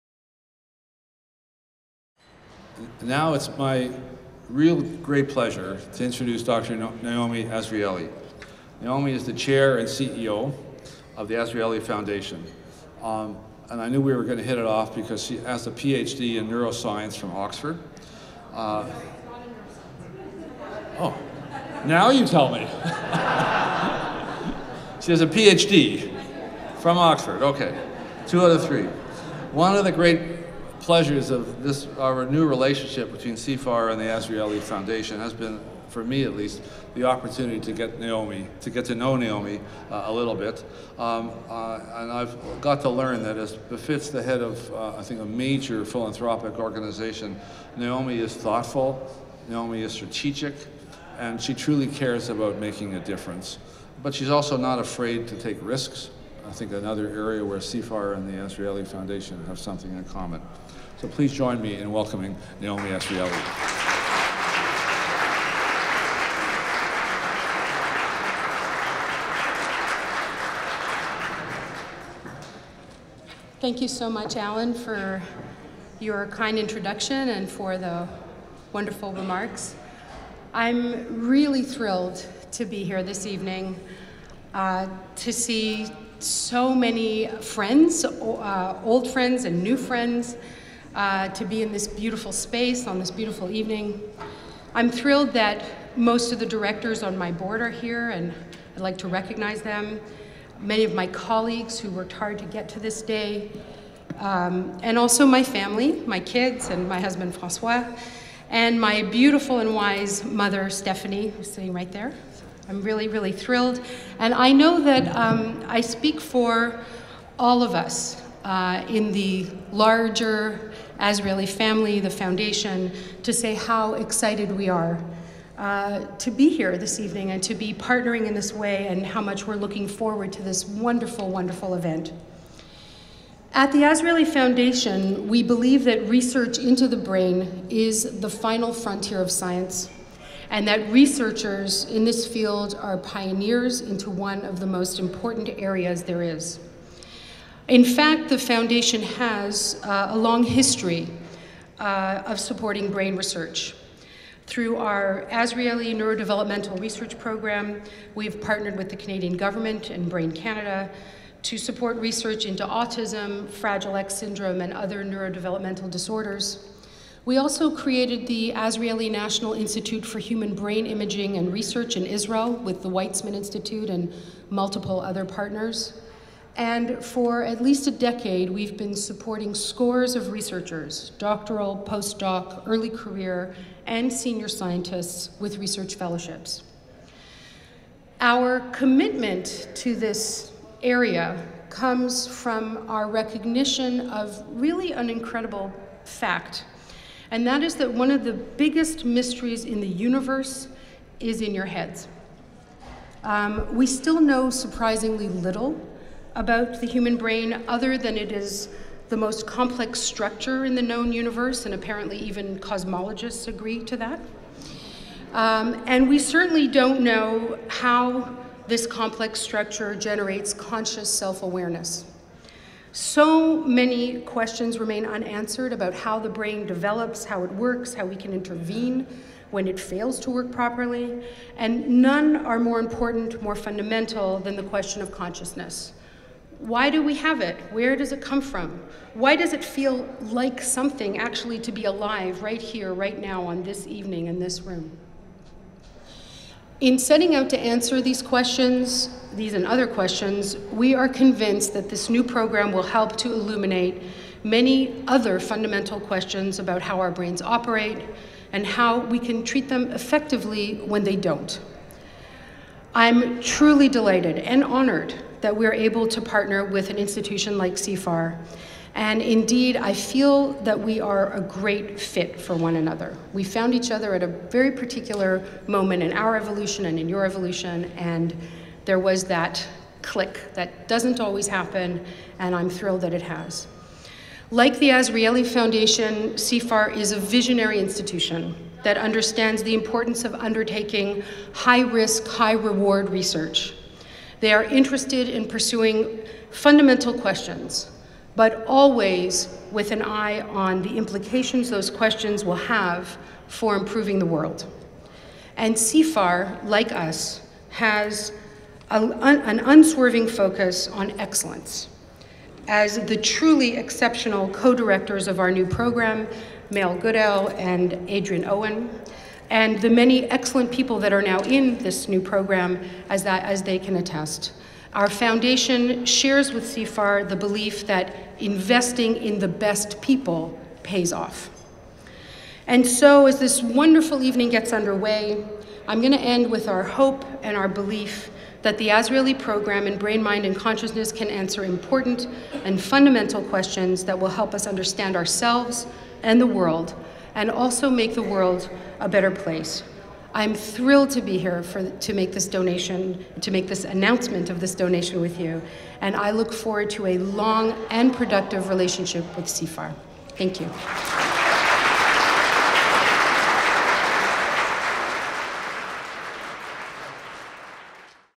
Our Musical Brain Azrieli Foundation Gift Reception - Naomi Azrieli remarks
Azrieli Foundation Chair & CEO Naomi Azrieli addresses the crowd at the Our Musical Brain Azrieli Foundation Gift Reception at Koerner Hall in Toronto, June 9, 2016.